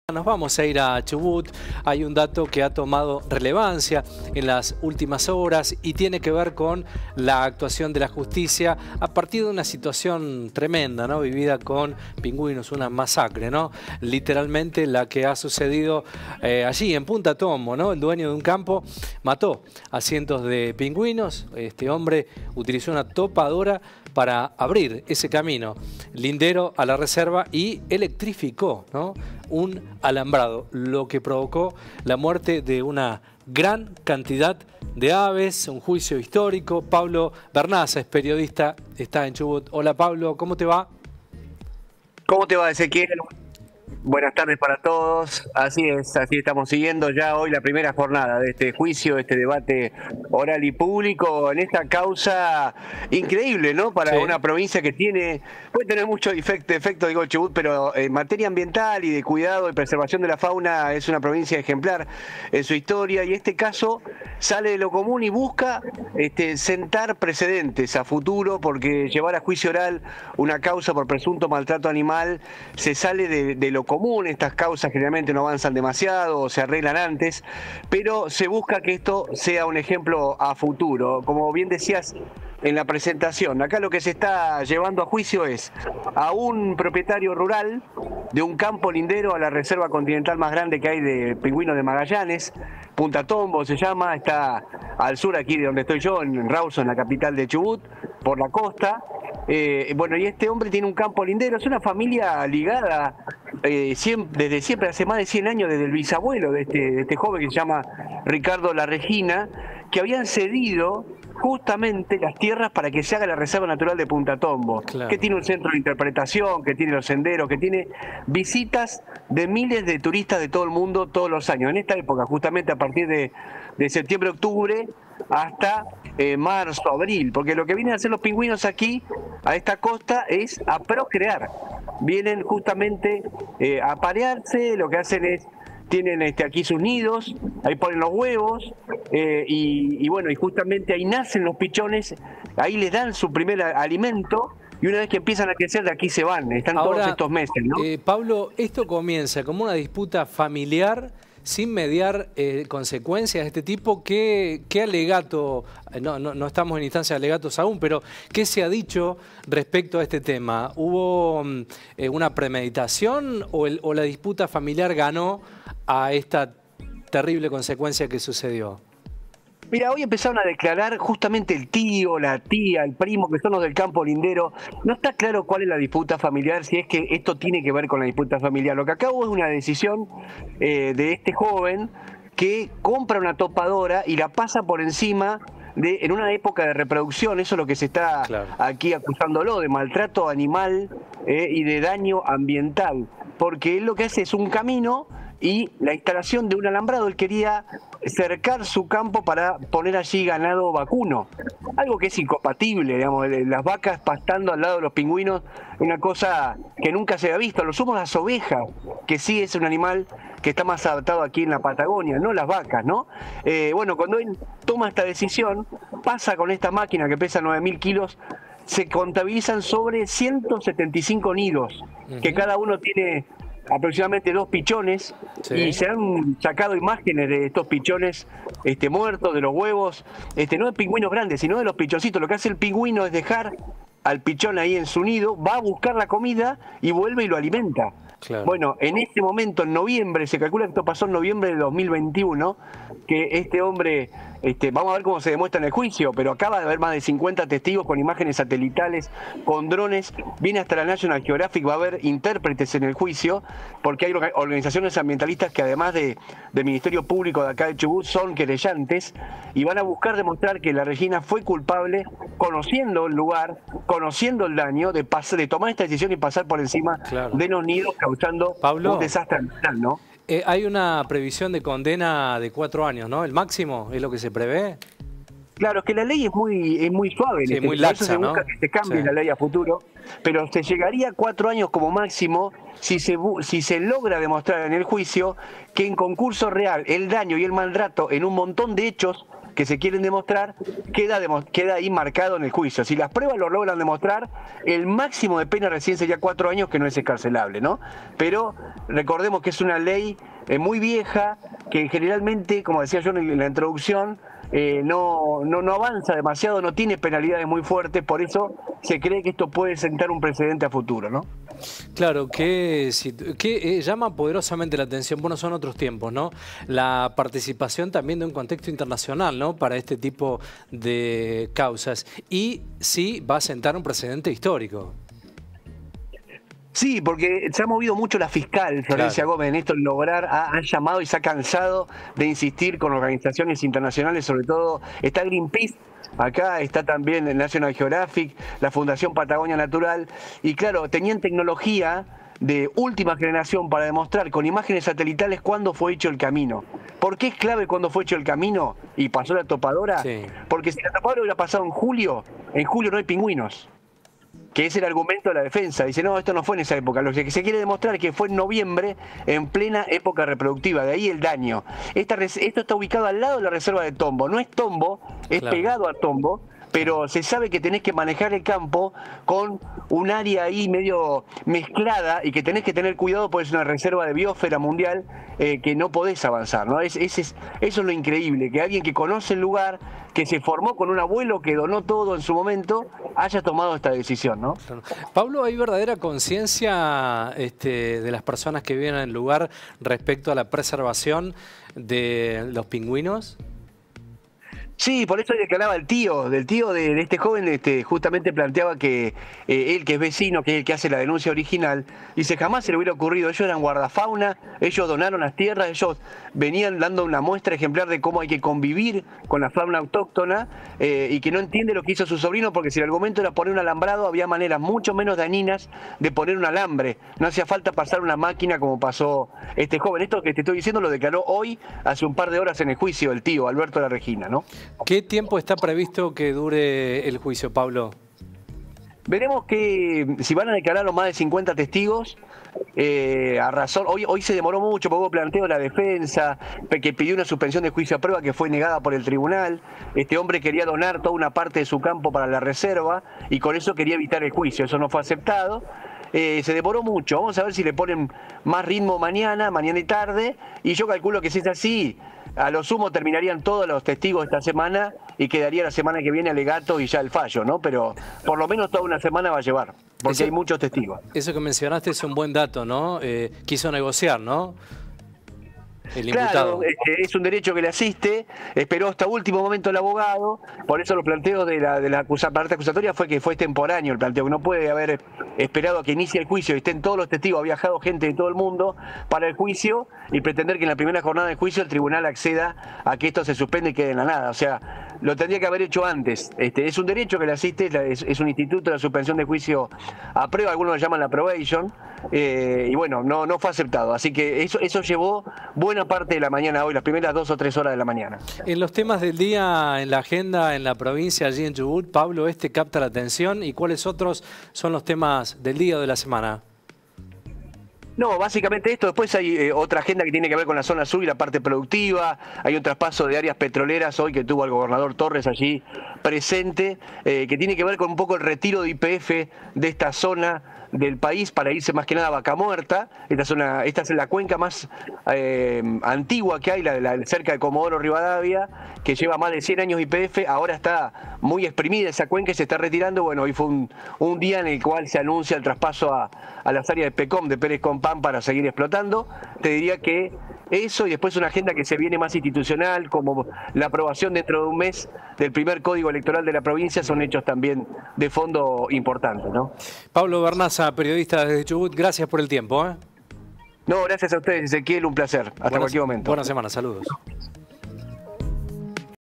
Juicio histórico: empieza el proceso por la masacre de pingüinos en la Patagonia
Dialogamos con Pablo Vernazza - Periodista de Chubut